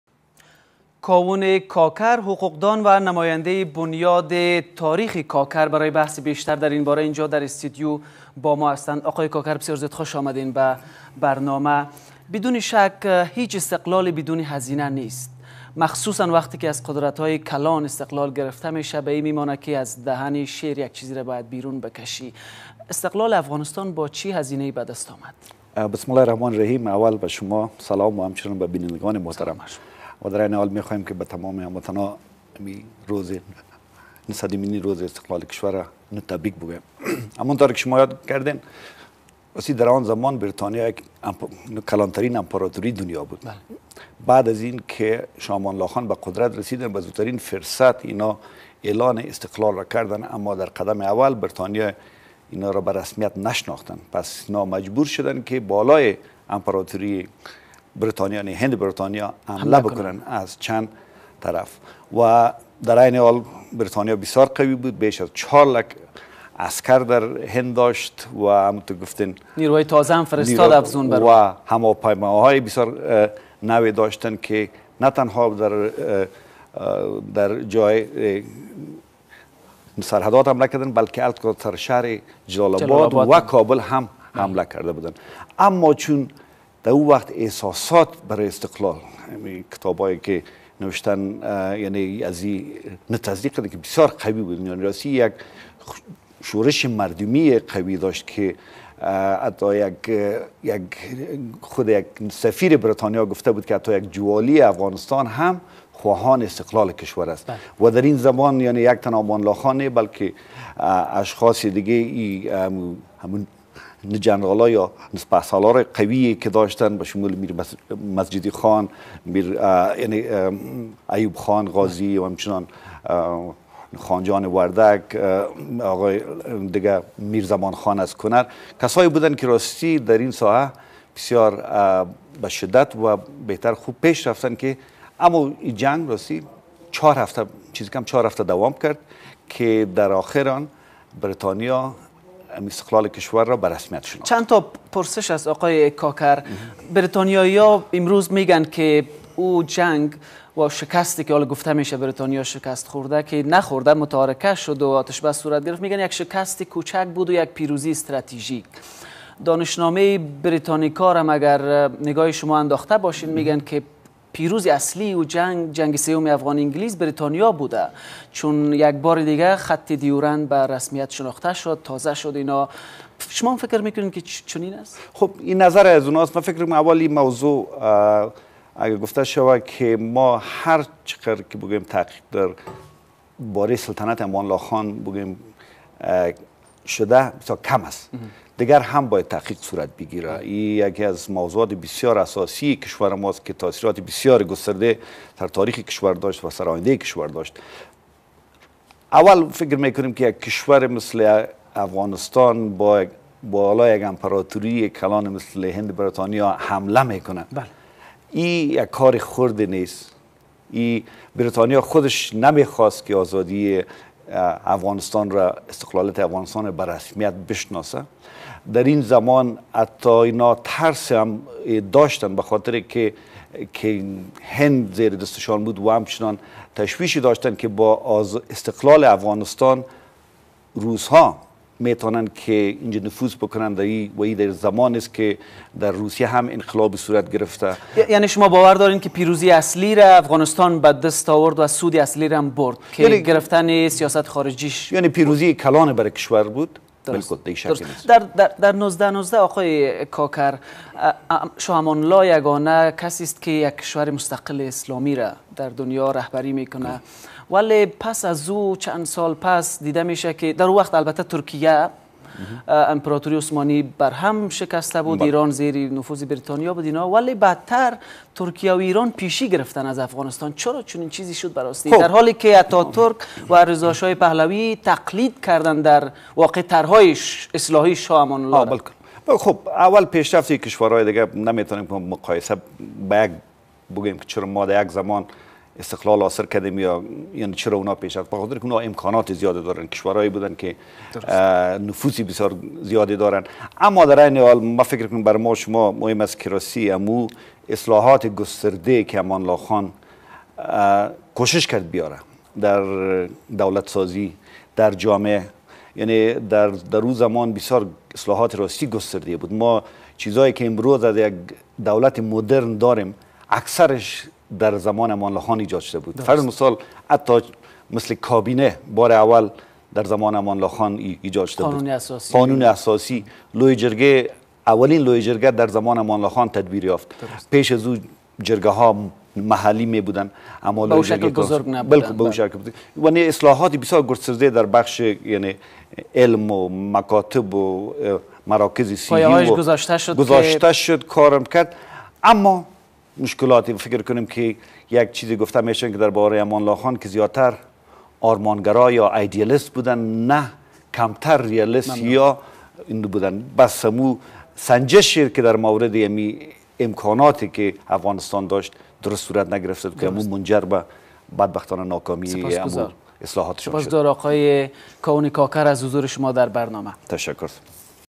مصاحبه محترم کاوون کاکړ با طلوع نیوز پیرامون سلطنت و اصلاحات شاه غازی امان الله خان
مصاحبه مختصر محترم کاوون کاکړ با طلوع نیوز پیرامون سلطنت و اصلاحات شاه غازی امان الله خان به مناسبت صدمین سالروز استرداد استقلال افغانستان